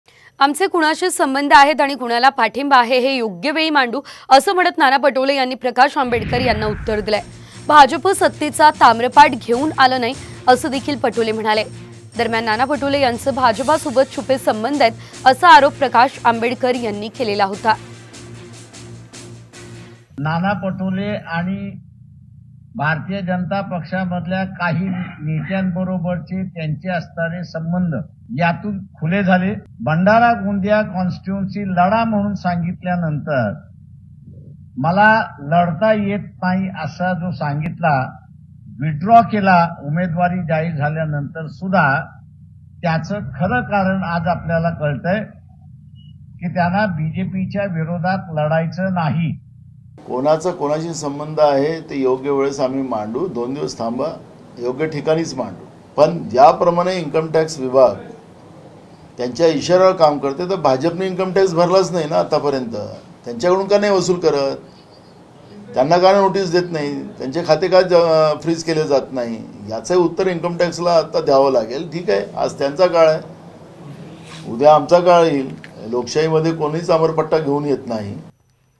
Nana Patole News | कुणाचा कुणाशी संबंध आहे आणि कुणाशी नाही हे योग्यवेळी मांडू नाना पटोले.
प्रकाश आंबेडकरांनी नाना पाटोले यांचे भाजपासोबत छुपे संबंध आहेत, असा आरोप केला होता यावर उत्तर देताना पटोलेंनी थेट स्पष्ट न करता कुणाचा कुणाशी संबंध आहे हे योग्य वेळी मांडू अश्या शब्दांत दिले, त्यांनी यावेळी माध्यमांशी बोलताना इनकम टॅक्स विभाग भाजपवर का लक्ष देत नाही, कुणीच सत्तेचा ताम्रपट्टा घेऊन आलेला नाही, अश्या शब्दात भाजपवर टिका केली.

Nana Patole News | कुणाचा कुणाशी संबंध आहेत आणि कुणाशी नाही हे योग्यवेळी मांडू अशा शब्दात नाना पटोलेंनी दिले प्रकाश आंबेडकरांना प्रत्युत्तर  Marathi News